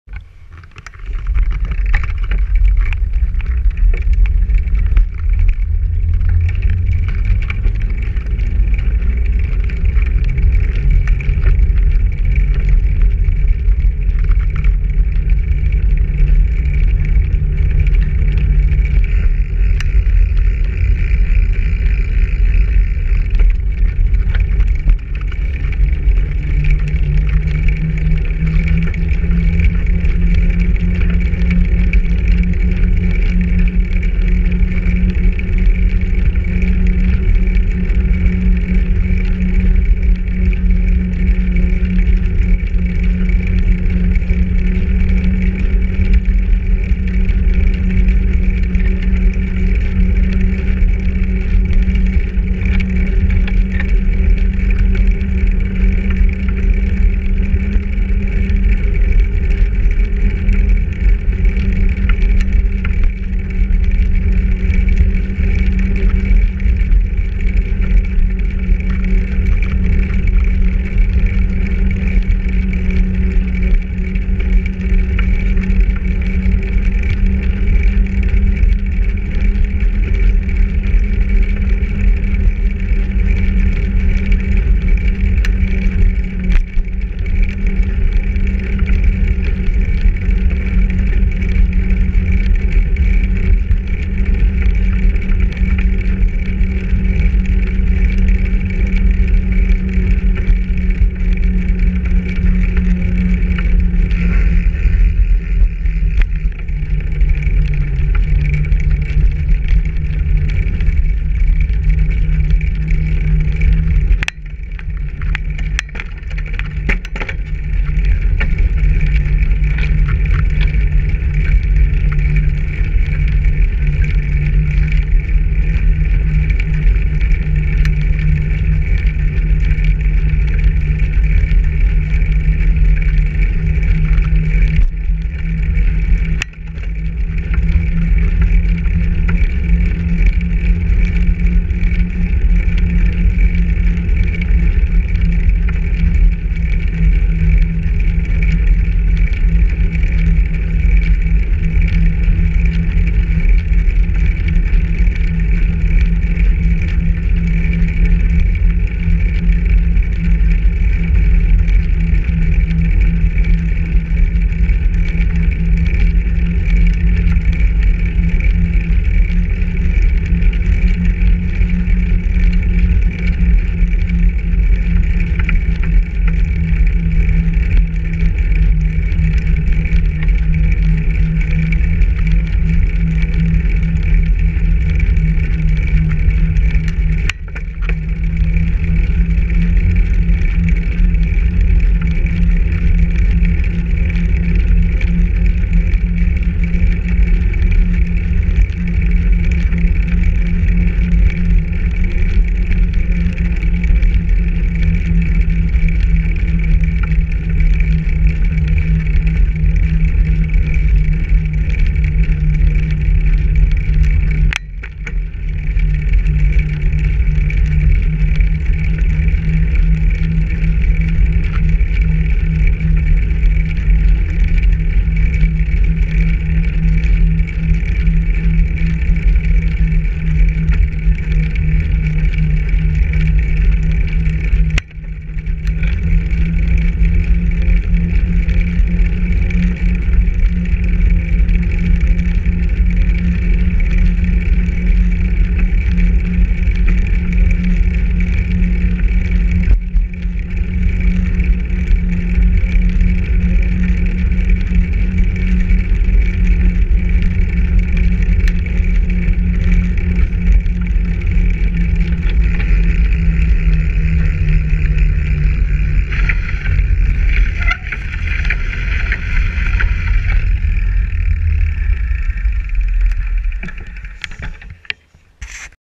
Testfahrt 1. Biken in Hausen. PART 3.
Erste Aufnahmeversuche mit meiner "Action Cam" in Hausen. Montage der Cam auf dem Lenkrad und bei 1080p bei 25 Frames. Cam mit offenem Visier. PART 3: Vom  Gewerbegebiet zur Firma Magna. Winter Januar 2014.